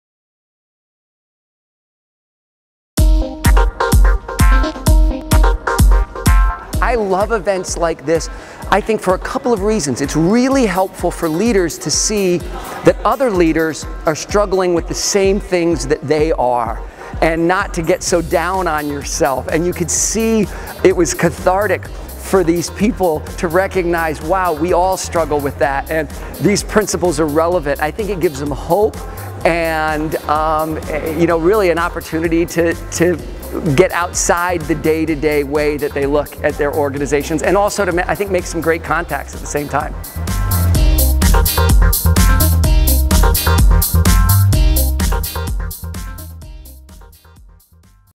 Business Transformation Summit 2018 - Testemunho Chris McChisney pós-evento
Chris McChesney ajuda organizações de todo o mundo a atingirem melhores resultados através de uma melhor execução das suas estratégias.

Chris McChesney é hoje o Global Practice Leader of Execution da FranklinCovey e um dos autores do livro “As 4 Disciplinas da Execução” (4DX). Chris tem sido o principal promotor da metodologia 4DX e, enquanto tal, há mais de uma década, um dos oradores mais solicitados em todo o mundo.

Consulte os nossos Blogs...